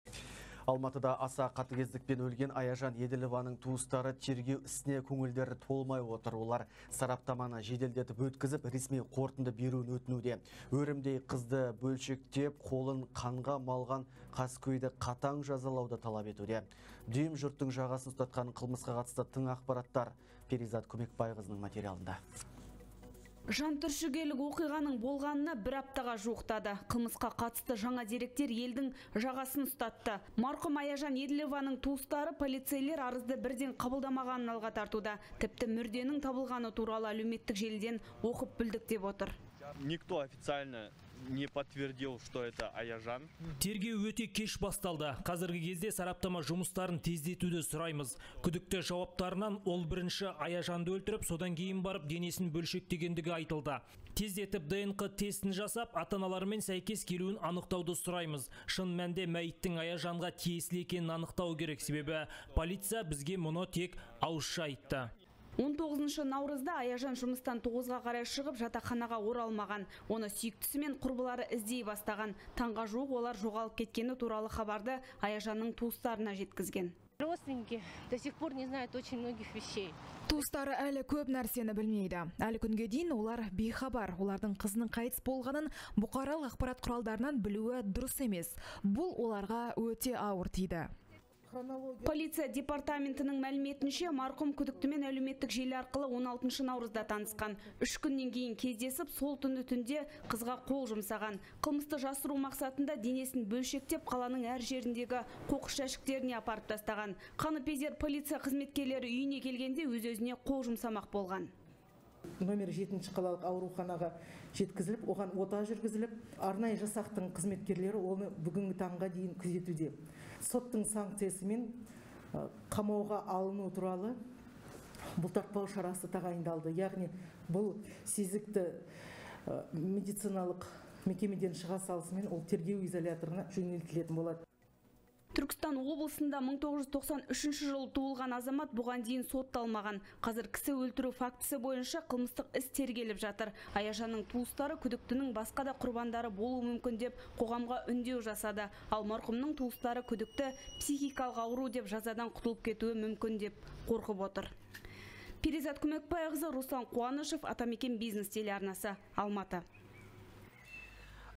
АЛМАТЫДА ҚАСКӨЙ ҚЫЗДЫҢ ДЕНЕСІН БӨЛШЕКТЕП, ҚАЛАНЫҢ ӘР ЖЕРІНЕ АПАРЫП ТАСТАҒАН
Дүйім жұрттың жағасын ұстатқан қылмыс. Алматыда аса қатыгездікпен өлтірілген Аяжан Еділованың туыстары тергеу ісіне көңілдері толмай отыр. Олар сараптаманы жеделдетіп өткізіп, ресми қорытынды беруін өтінуде. Өрімдей қызды бөлшектеп қолын қанға малыған қаскөйді қатаң жазалауды талап етуде.
 Жантүршігерлік оқиғаның болғанына 1 аптаға жуықтады. Қылмысқа қатысты жаңа деректер елдің жағасын ұстатты. Марқұм Аяжан Еділованың туыстары полицейлер арызды бірден қабылдамағанын алға тартуда. Тіпті мүрденің табылғаны туралы әлеуметтік желіден оқып білдік деп отыр.